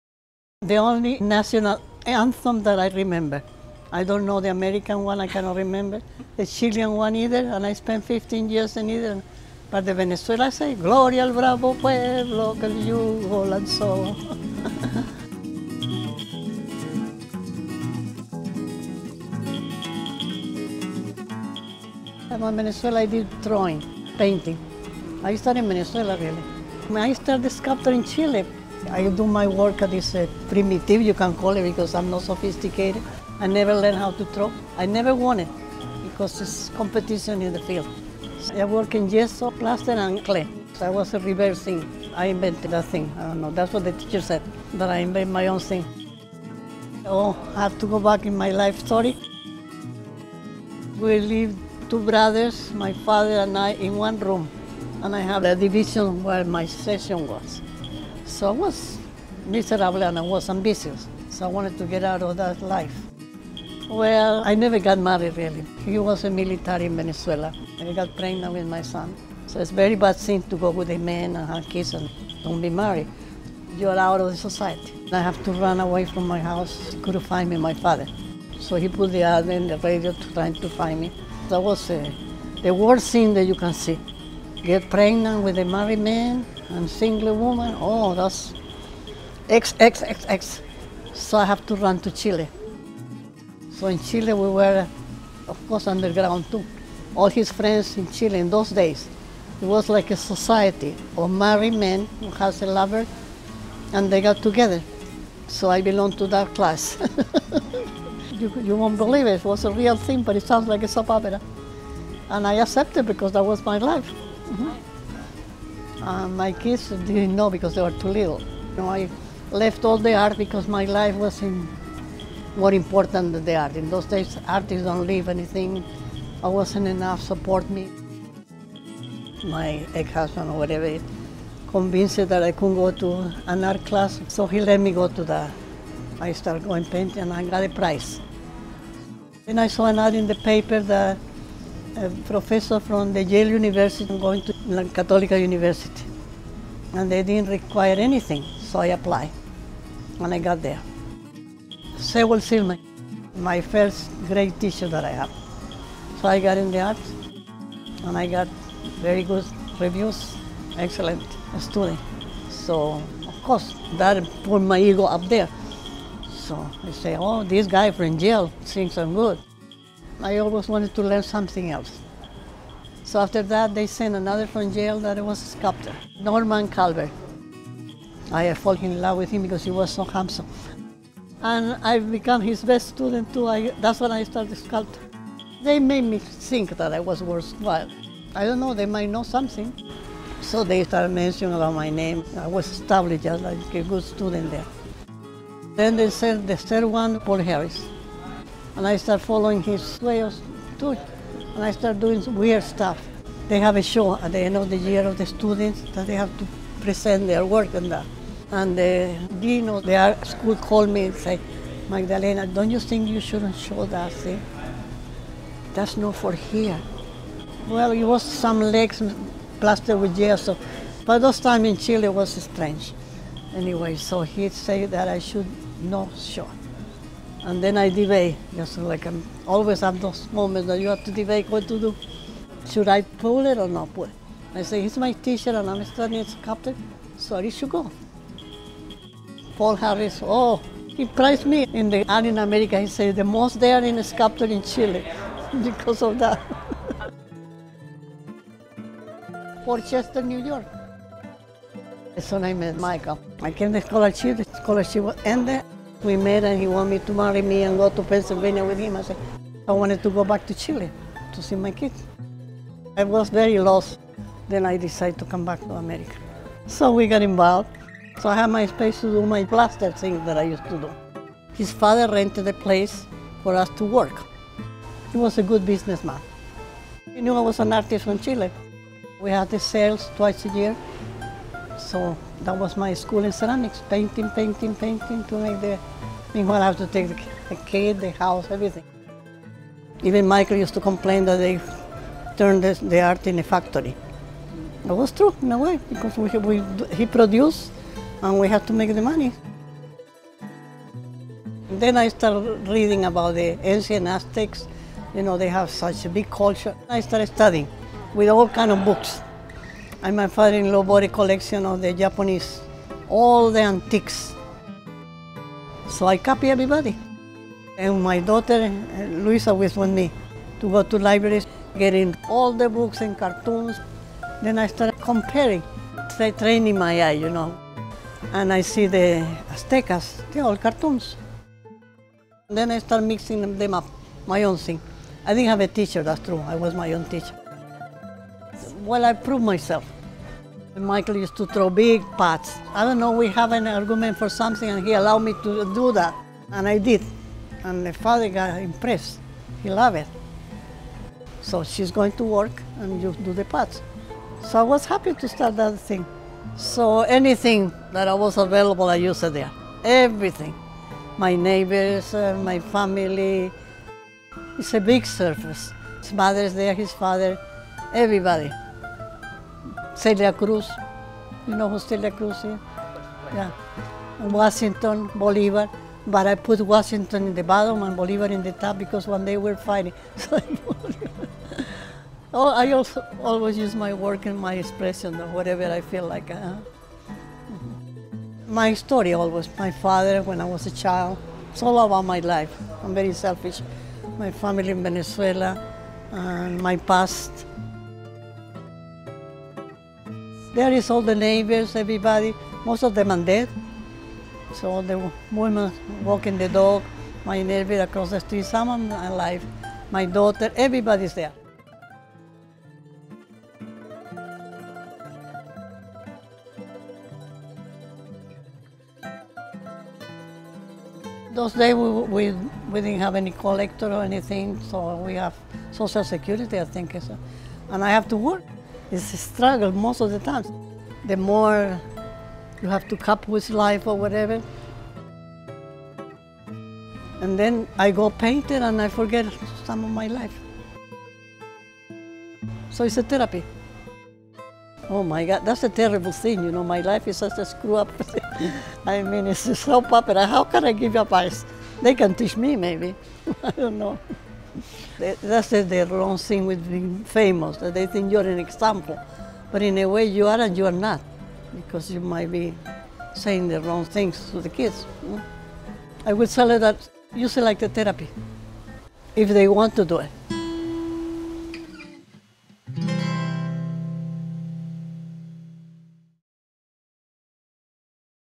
Artist Portrait: Magdalena Suarez Frimkiss
Magdalena Suarez-Frimkiss is an artist and sculptor living in Venice, California. Her work spans 7 decades and features playful images of pop culture and personal stories on classically shaped ceramic vessels.
Produced and Edited by Debra Koffler. 2021.